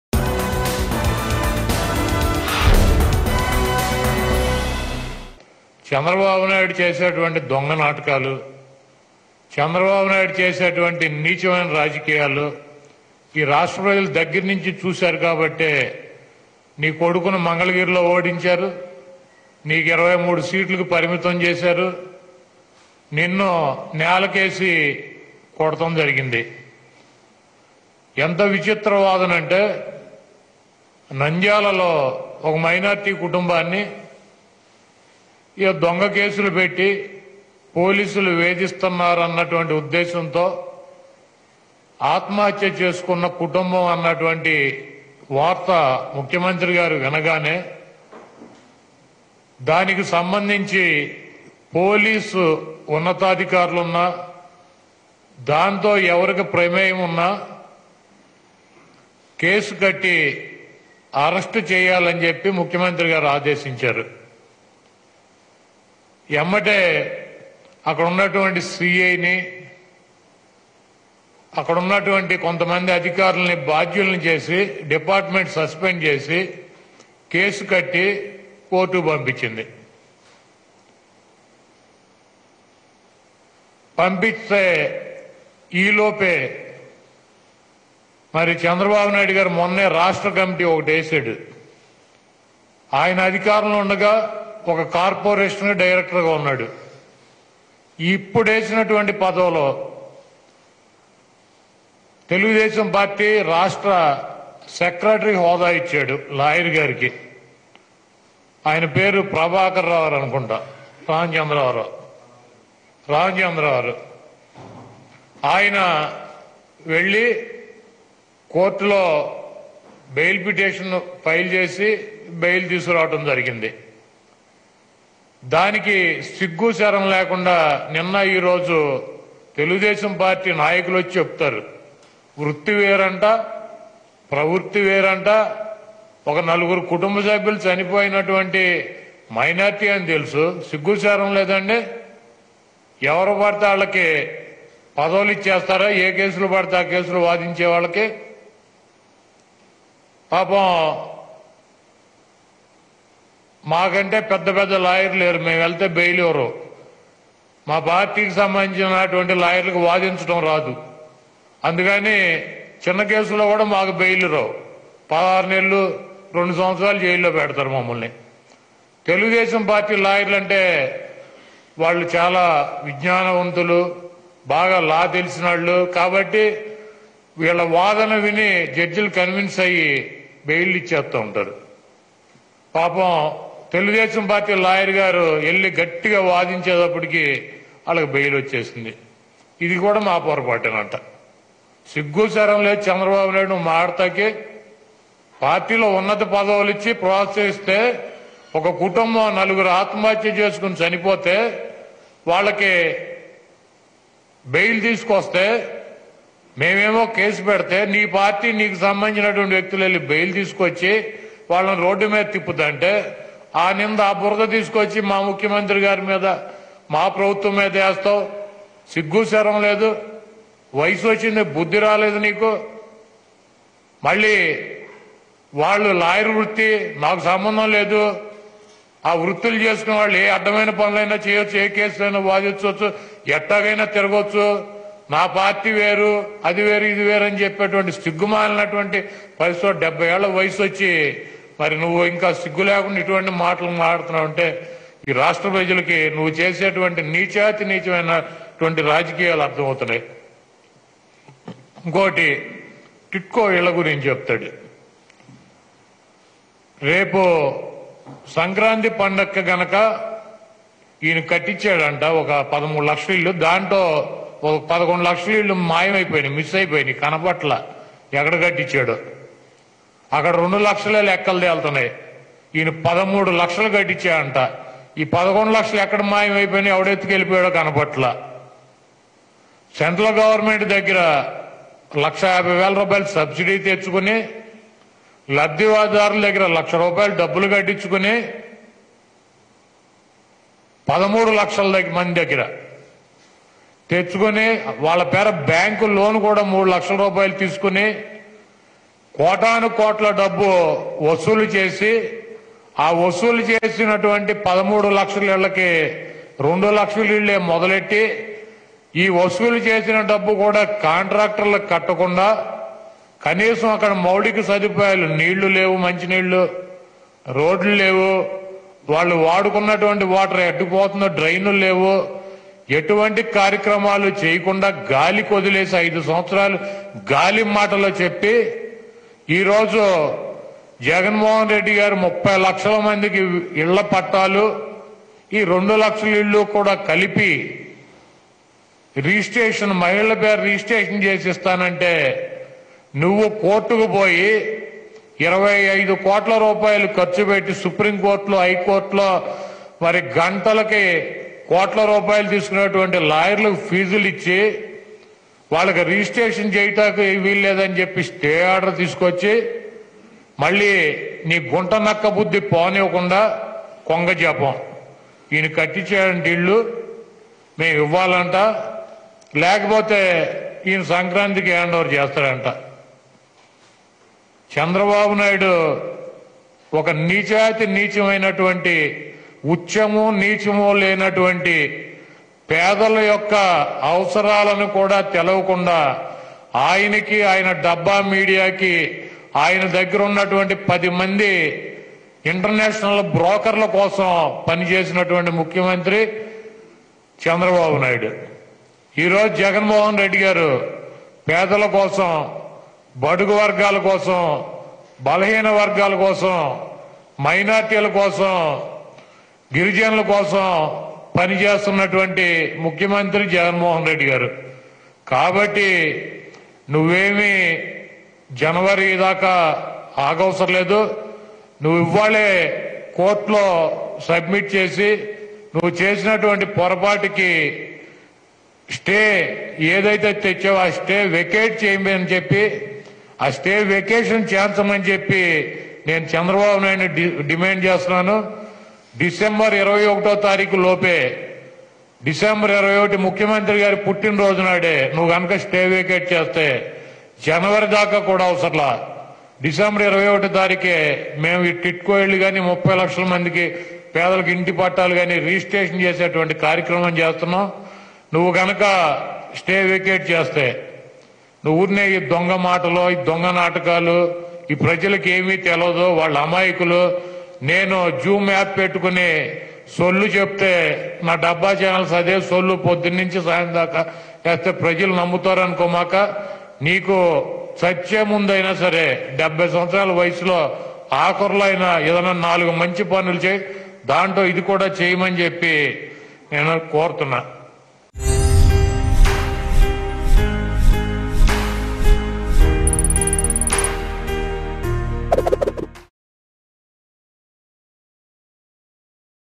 Kodali Nani Fires On Chandrababu Naidu Over Nandyal Incident In Press Meet | AP News | Mango News
Kodali Nani Fires On Chandrababu Naidu Over Nandyal Incident In Press Meet